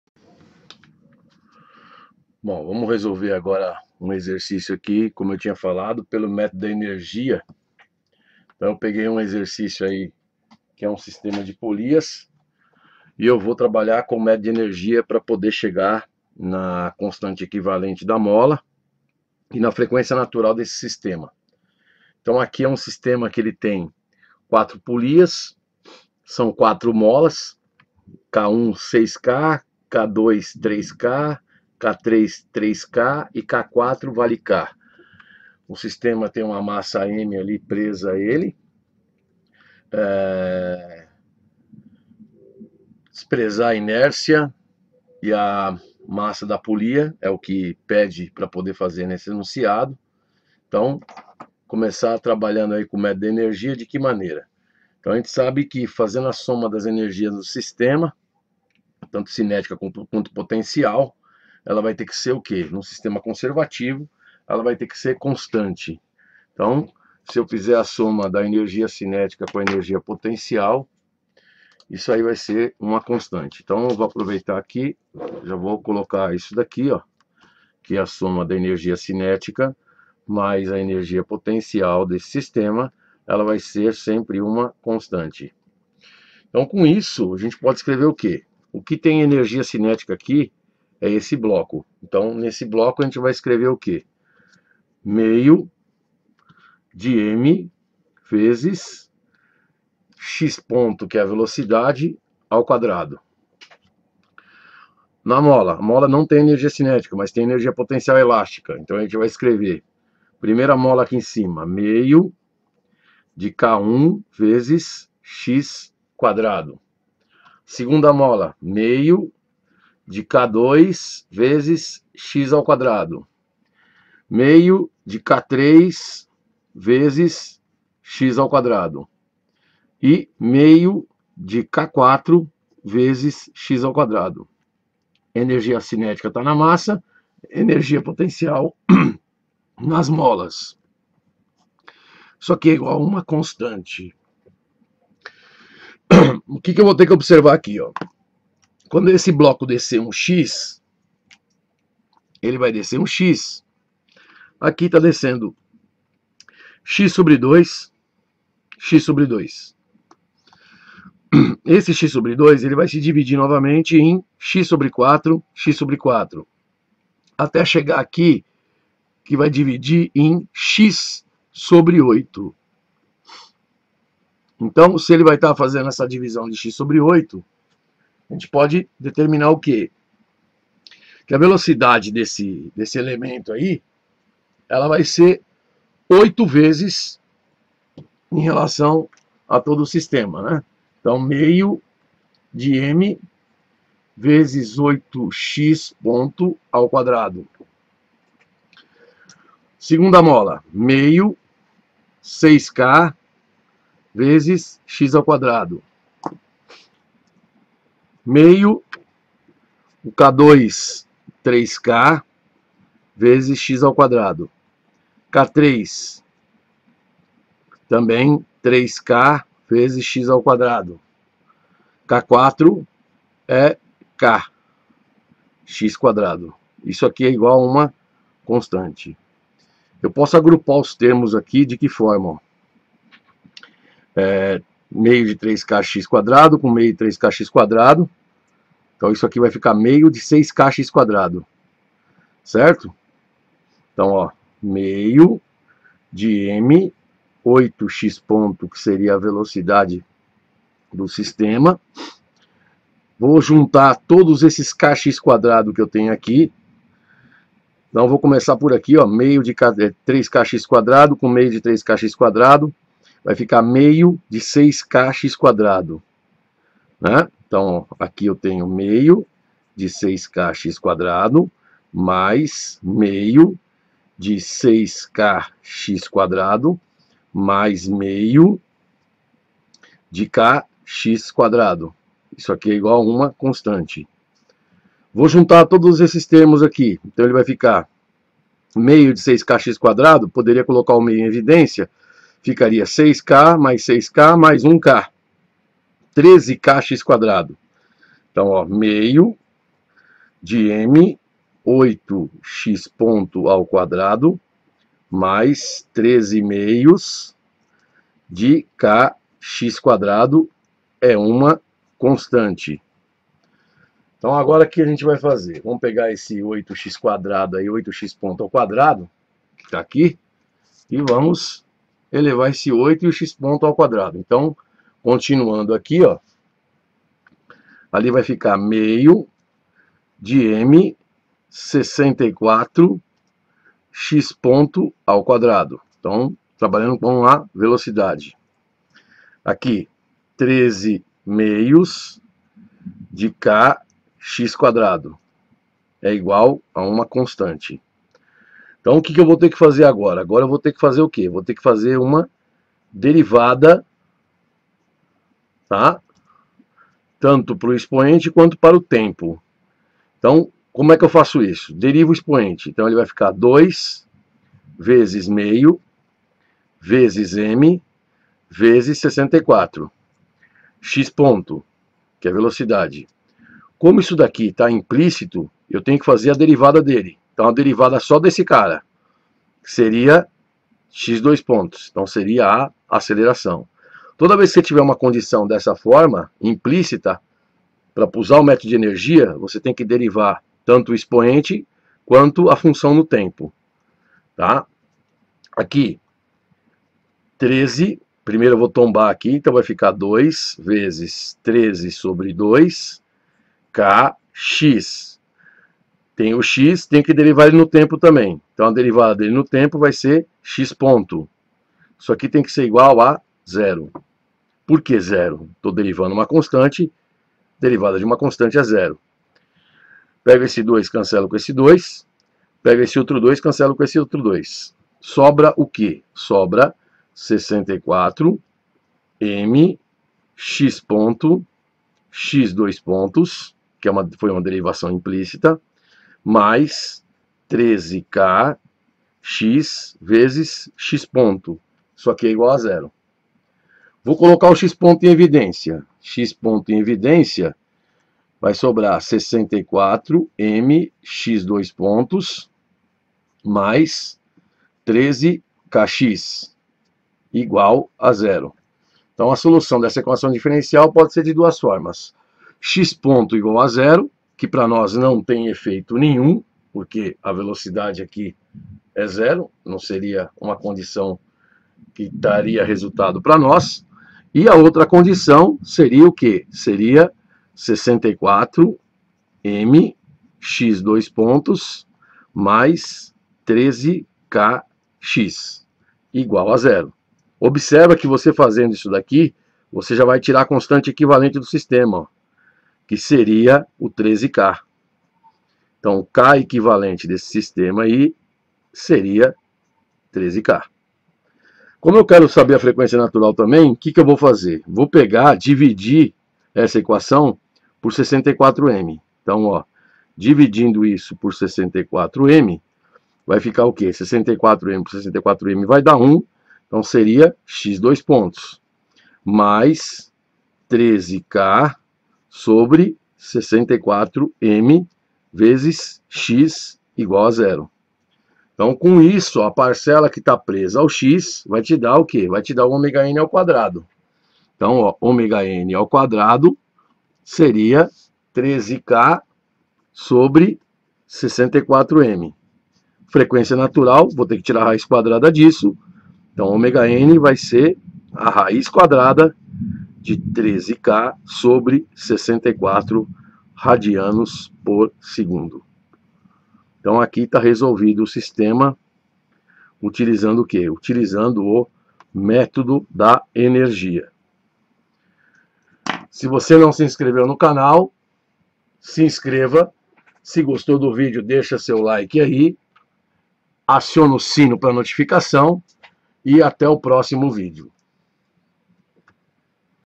Vibrações mecânicas método da energia episódio 3
Neste vídeo é apresentado um sistema de polias onde há a necessidade de determinar a constante equivalente de mola e a a sua frequência natural utilizando o método da energia.